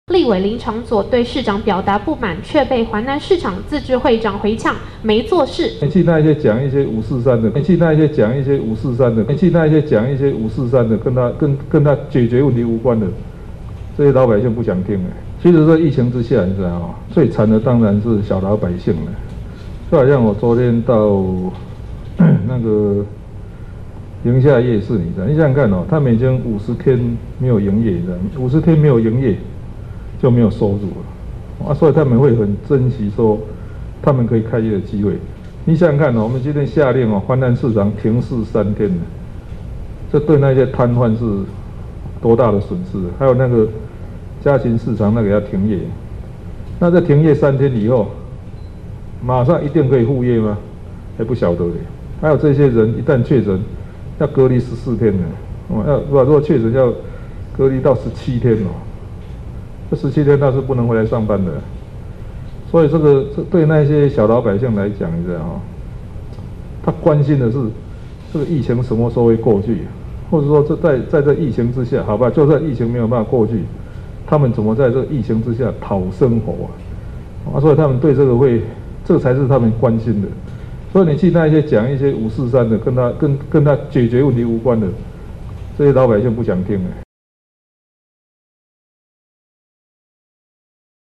林昶佐嗆柯反遭攤商洗臉 柯P：講543老百姓不想聽
立委林昶佐今(2)日赴環南市場，卻遭攤商連番嗆聲。對此，台北市長柯文哲在防疫記者會上表示，疫情之下最慘的是小老百姓，他昨天到寧夏夜市視察，攤商50天沒有收入，相當珍惜開業機會。今天下令環南市場休市3天，對攤販來說損失慘重，能不能馬上復業也不知道，且確診後也要被隔離17天不能上班。老百姓最關心的，就是疫情什麼時候會過去，或是疫情之下要如何討生活，如果只是講一些543的，和解決事情無關，老百姓不想聽。